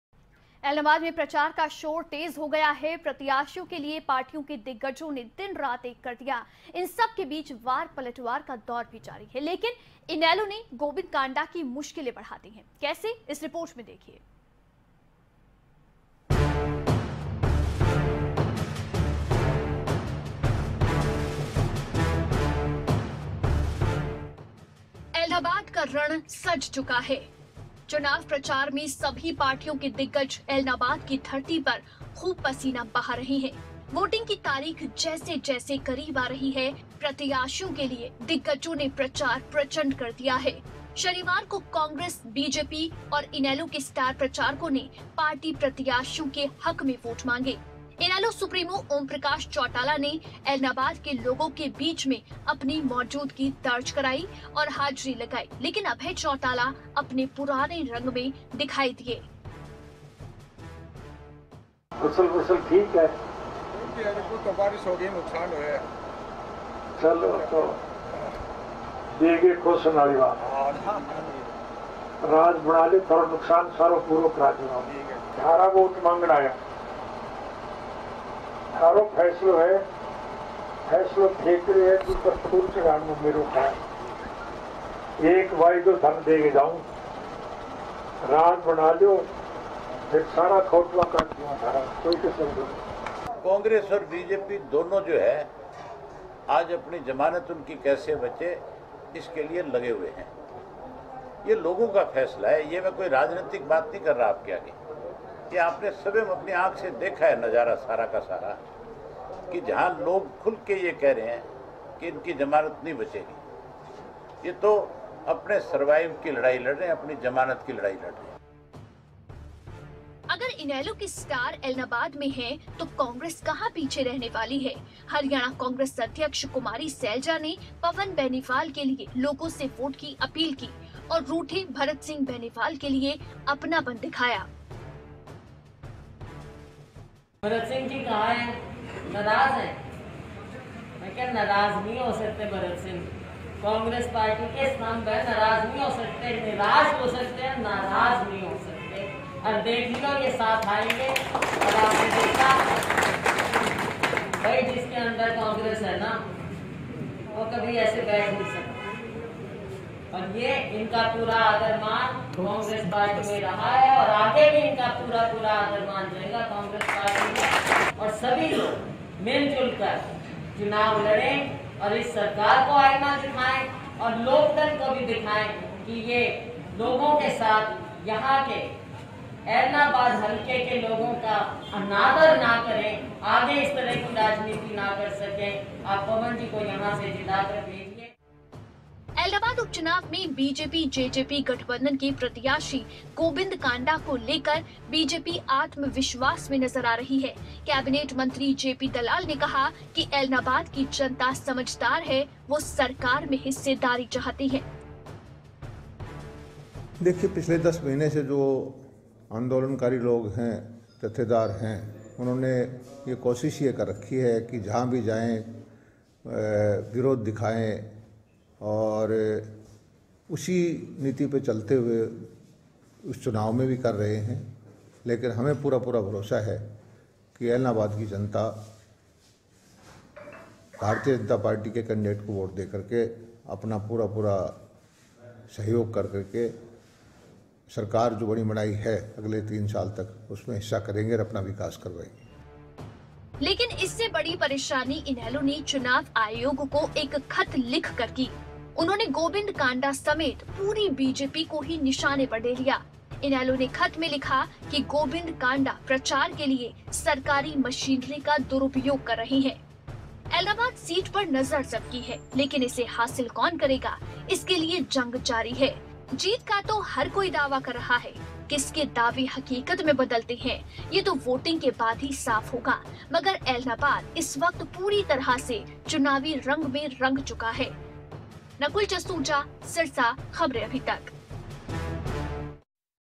Ellenabad By Polls: INLD का BJP उम्मीदवार पर गंभीर आरोप, क्या गोविंद कांडा की बढ़ेगी मुश्किलें ?
Ellnabad By Polls: INLD का BJP अम्मीदवार पर गंभीर आरोप, क्या गोविंद कांडा की बढ़ेगी मुश्किलें ?